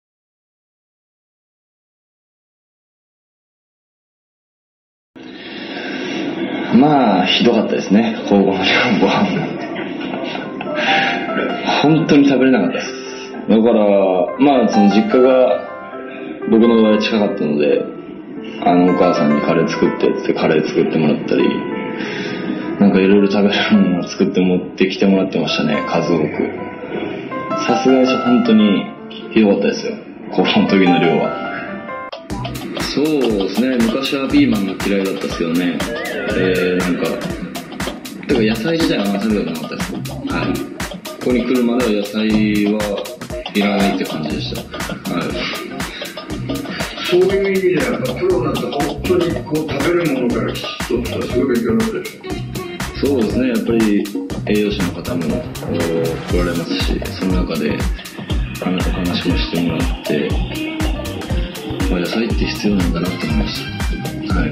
今宮健太が語る明豊高校時代の食生活ｗ
↑↑今宮健太と柳田悠岐の秘話↑↑

福岡ソフトバンクホークスのパ・リーグを代表するショートストップいまみやけんた選手の明豊高校時代のエピソードインタビューです。過去の彼は、随分と偏食だったらしく、寮生活が大変だたようです。また、実家の母親によく手料理をつくってもらっていたといいます。嫌いな食べ物がピーマンとは意外ですｗ